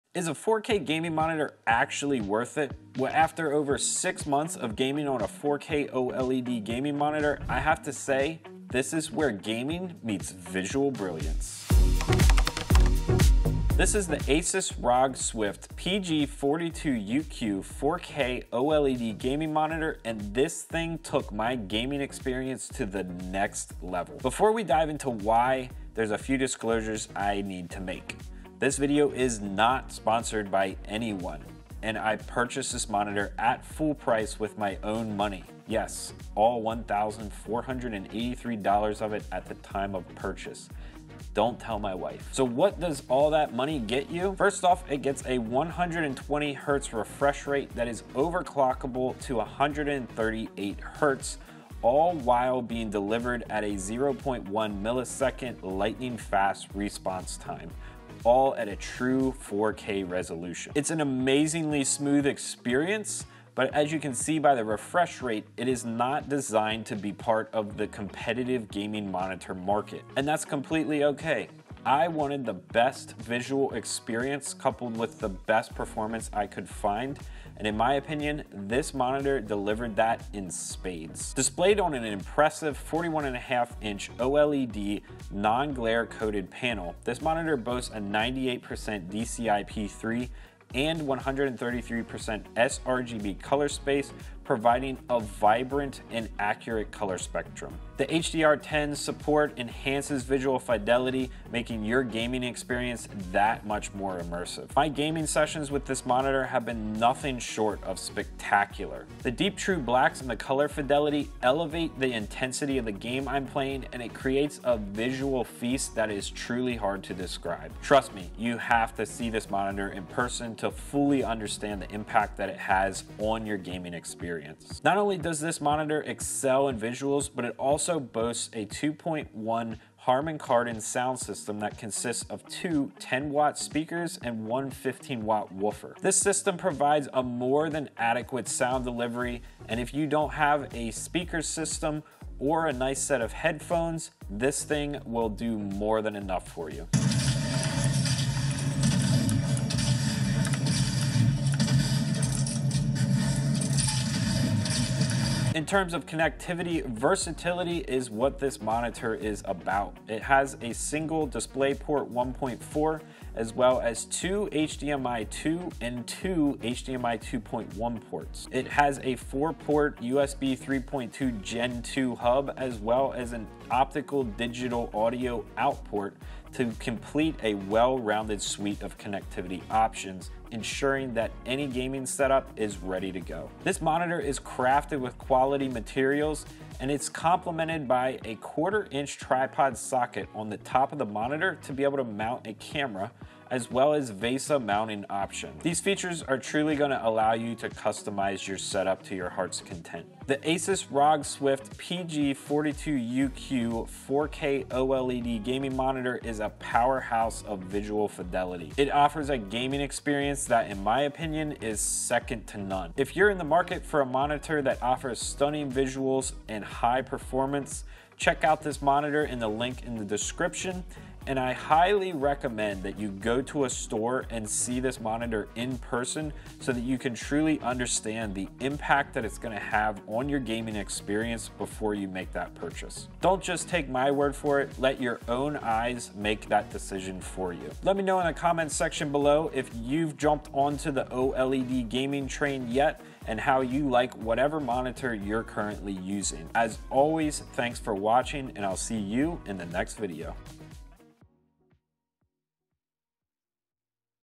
Is 4K OLED gaming worth it? - Asus ROG Swift PG42UQ
Is 4k OLED gaming worth it? I take a look at the ASUS ROG Swift PG42UQ and help you decide if 4k OLEG gaming worth it. The Asus RGO Swift PG42UQ is one of the best OLED gaming monitors on the market and delivers incredible visuals.

Check out the monitor below!

41.5" Version:

47.5" Version: